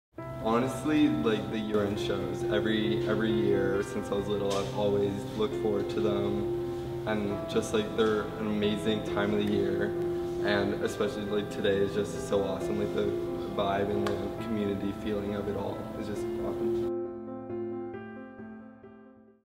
Dance With Us at Dance Vancouver - Our Year End Show! | The Landing Dance Centre
Ben from our 2018/2019 Senior Company, tells us why he loves our year end show- Dance Vancouver!